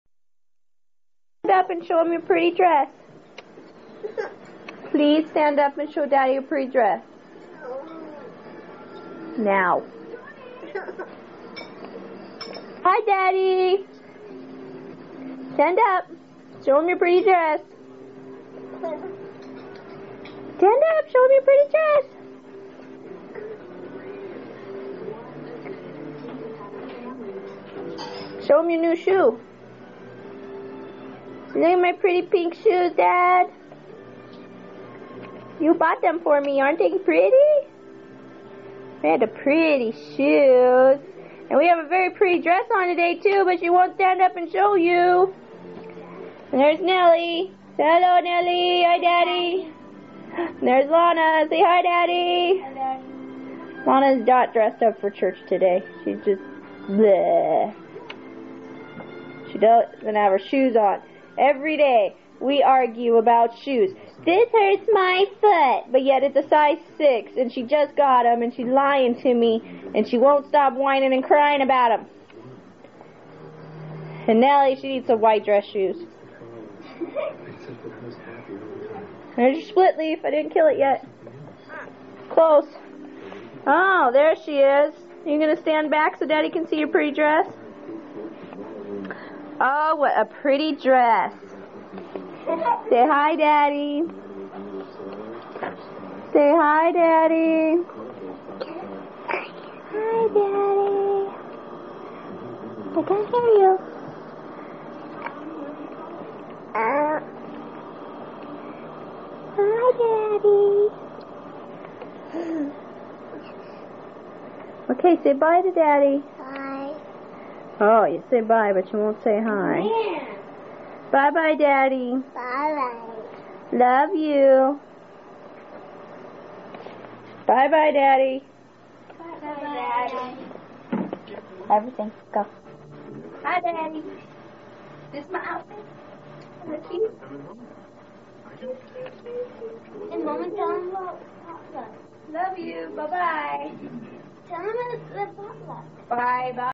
Polly & the girls before church
Getting ready to head to church one morning, Polly took out the camera to get some footage of our girls while I was away working in an oil refinery.